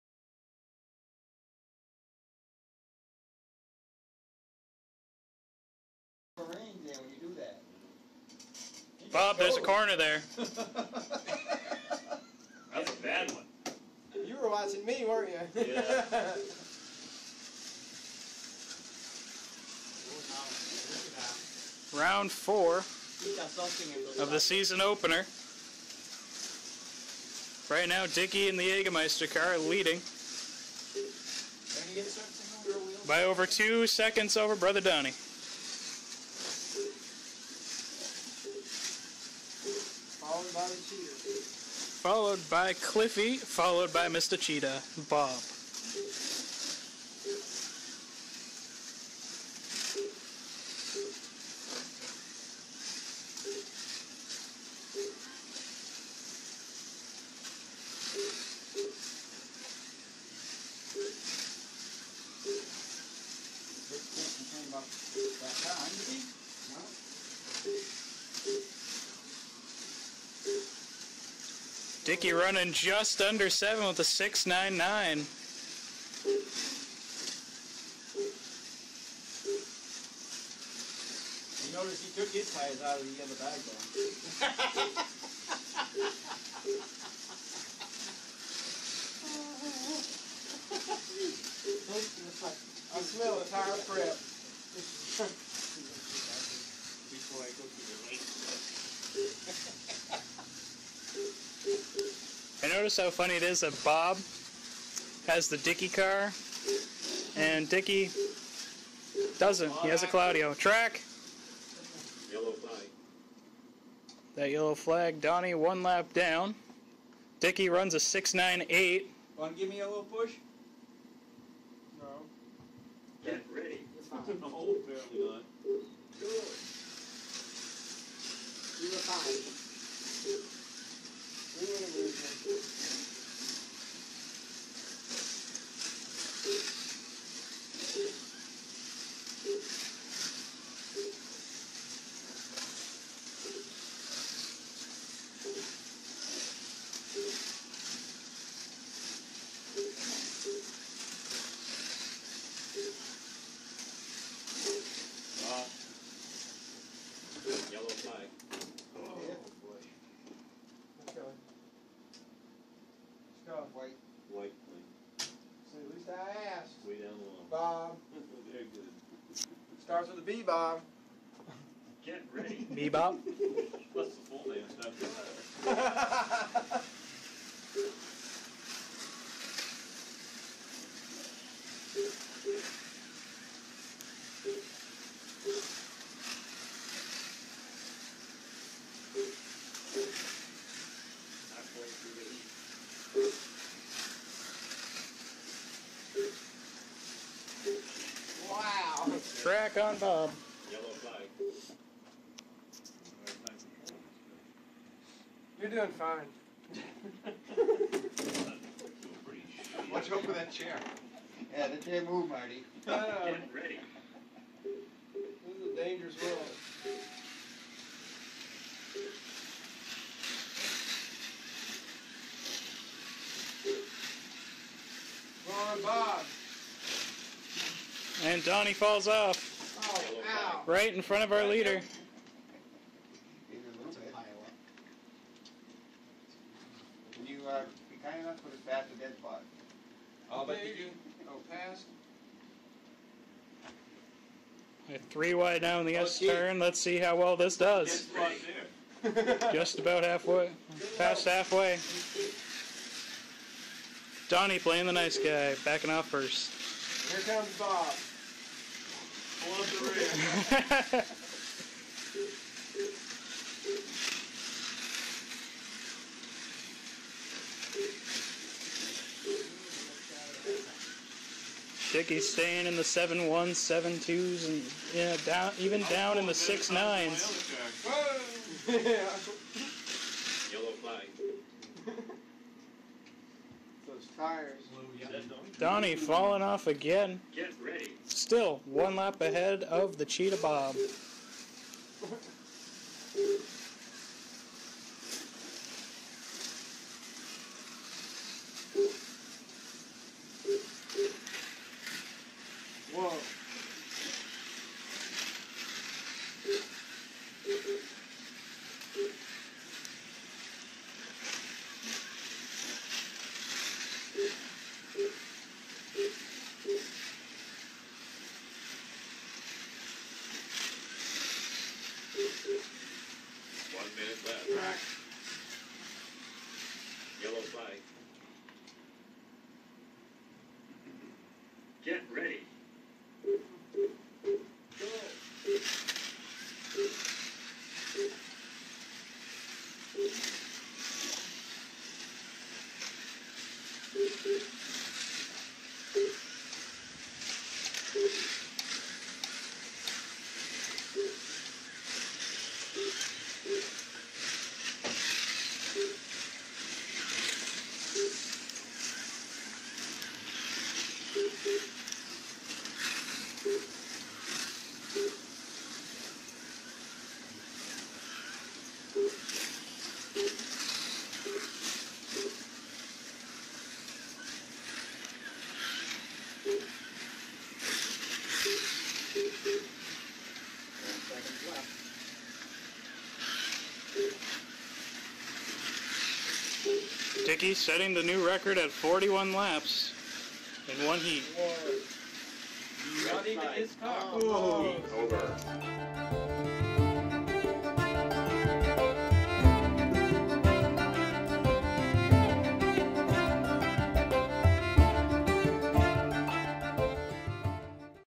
Group C SSC Opener
Group C action begins at Slot Car Corner, this is just one clip of the 9 heat racing that was done during the night.  9 brave souls came out to the store to face off, with the winner coming home with plenty of points.  The overall winner will be decided at the end of the season to the person with the most laps over the span.  We ran 5 minute heats with flags being called for falling out of the slot.  Interested in Slot Car racing after watching this video? check out a local track rent a car for a time and even get your very own!